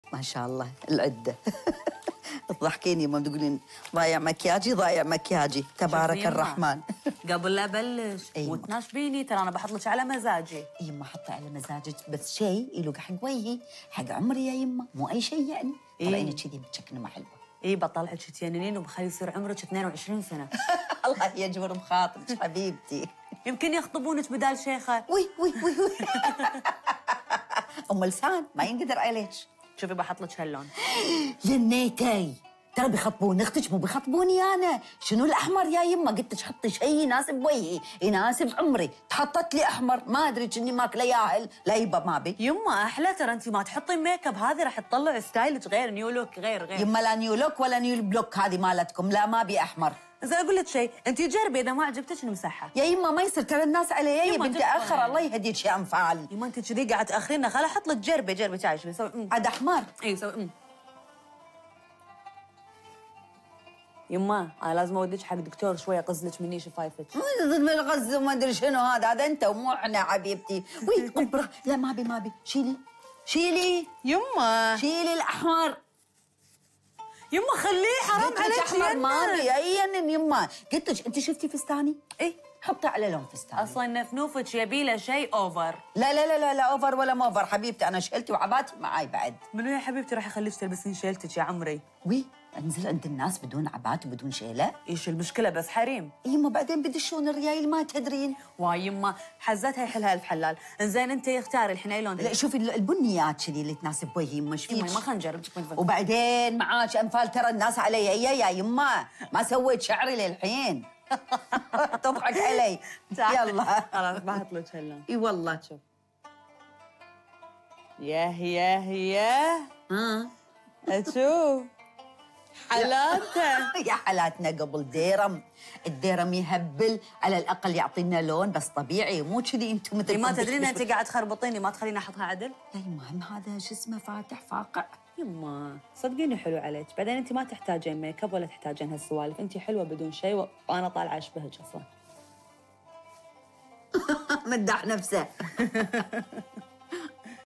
بطلعك تجننين وعمرك 22 سنة.. مشاغبات أنفال وهي تضع المكياج لأمها
🔔 Stay updated!